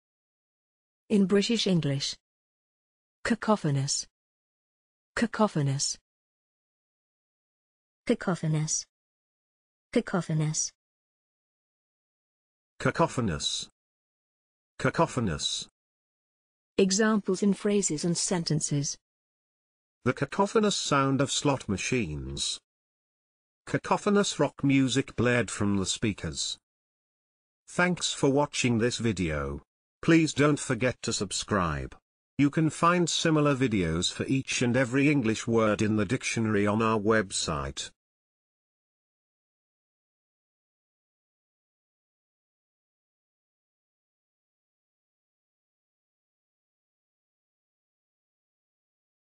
cacophonous - pronunciation + Examples in sentences and phrases
This is the pronunciation of cacophonous in British English + a few examples of its pronunciation in phrases and sentences.